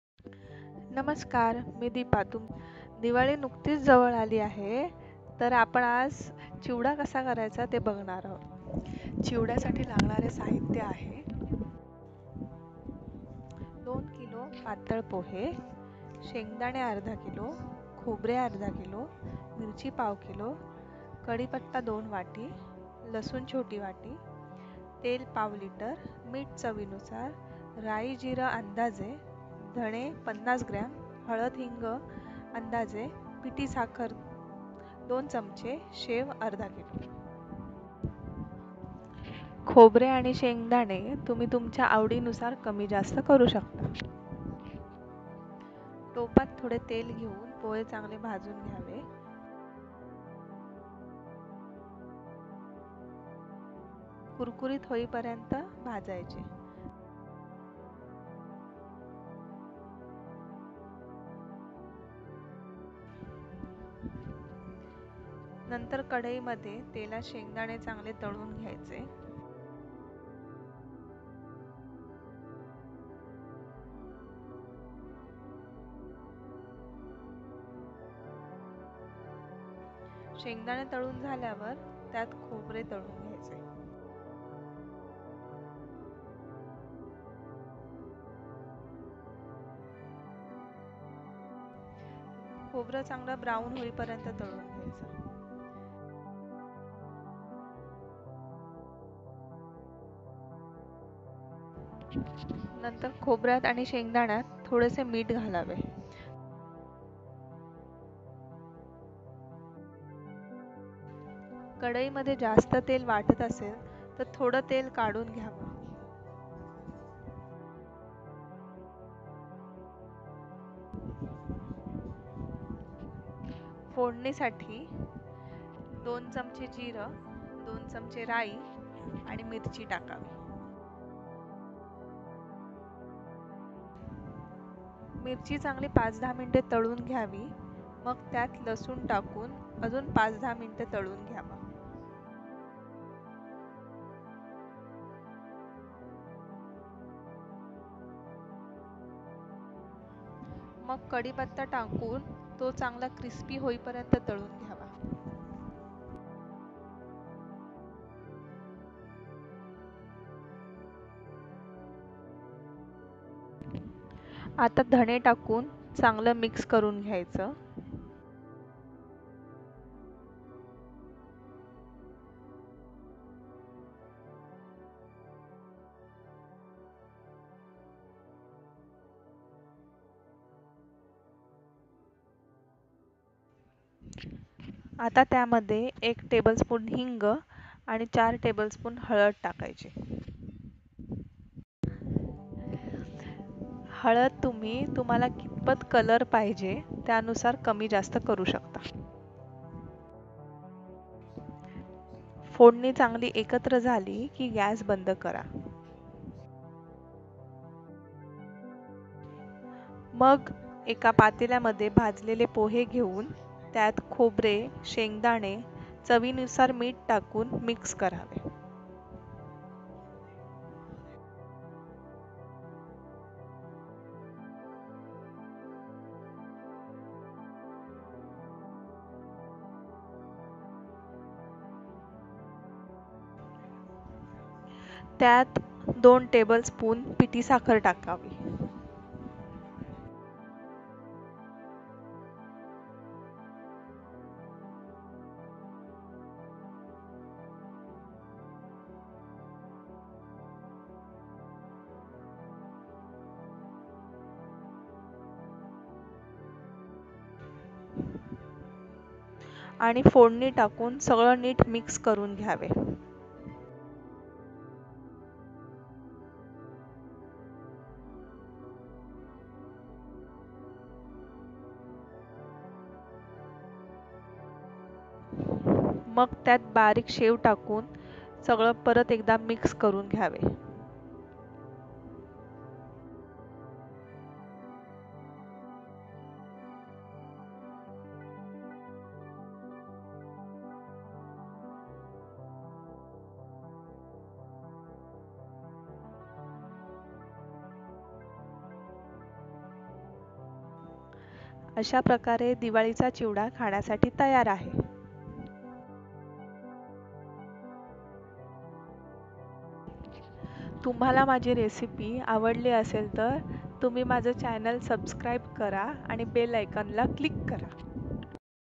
दिवाळी स्पेशल पोह्यांचा चिवडा | Diwali Special Chivada | Pohe Chivada Recipe
Use this recipe to make delicious crunchy chivada for this Diwali